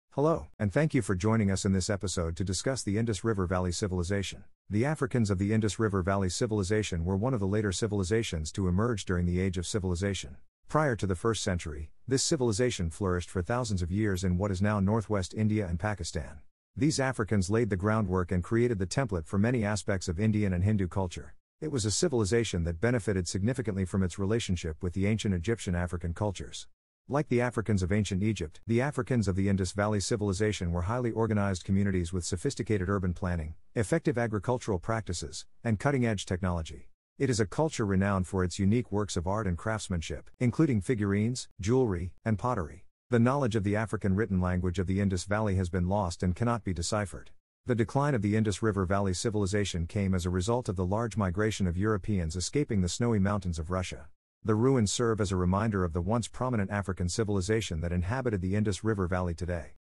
Indus Valey Civilization
One of the late civilizations to have emerged in the age of civilization was the Africans of the Indus River Valley.

Today, their way of life is still retained in our homes, in the way we farm and use machinery, and in the way we worship and educate.